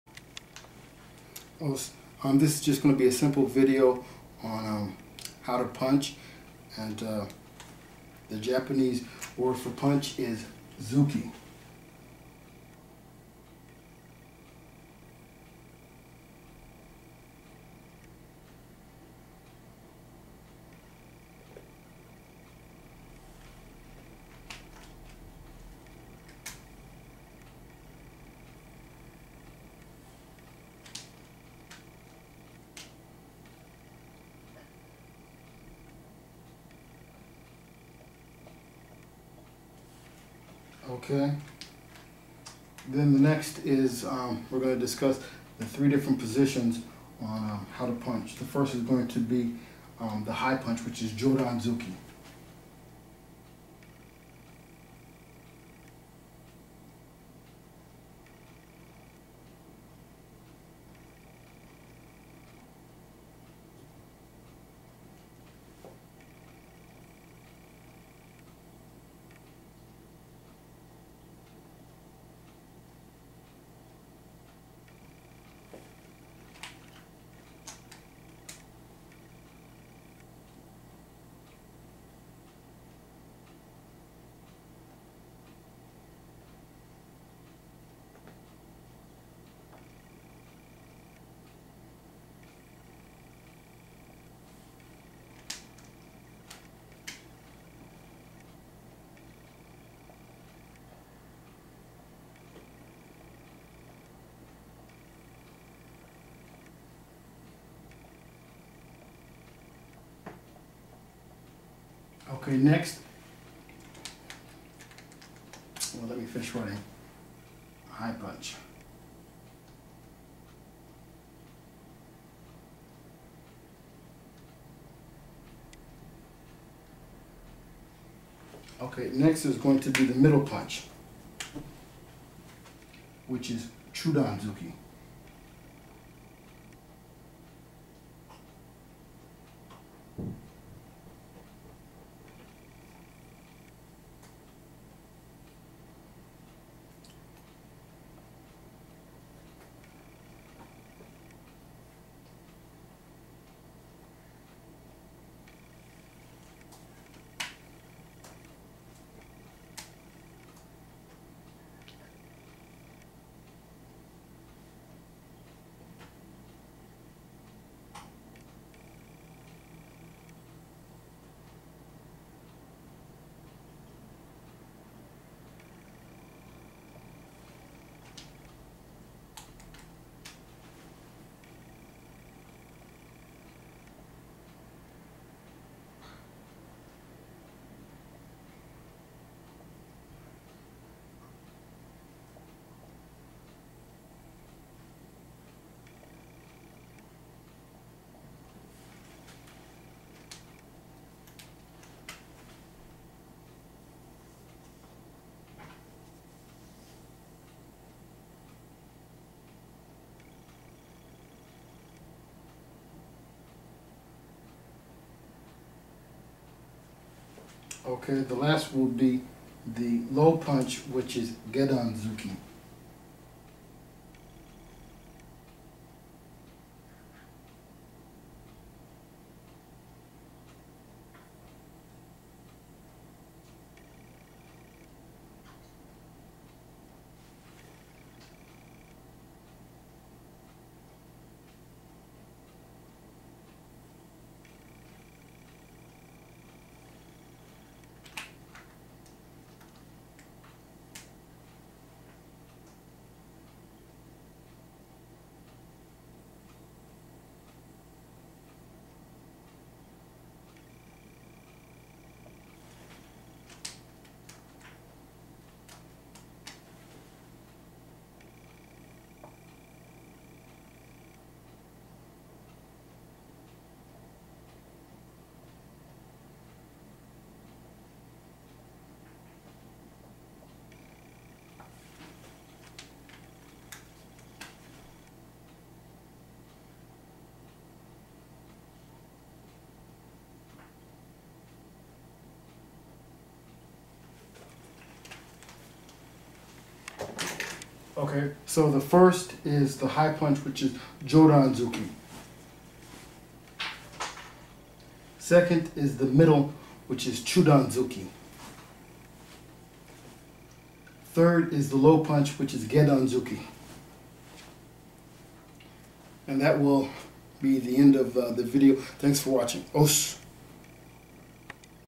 A Tutorial on the Japanese Martial Arts Technique: Sei Ken Zuki "Punch"
Ho Oo Ryu presents a tutorial on the Japanese Martial Arts Technique Sei Ken Zuki. In this vidoe the Japanese writing system of Kanji, Furigana and Romaji will be utilized when illustrating the High(Jodan), Middle(Chudan) and Low( Gedan) punch (Zuki/Tsuki). At the end of the lecture section of this video a brief demonstration of the three techniques discussed above will be performed. Note: the Japanese word for punch is TsuKi when it is not used with other words.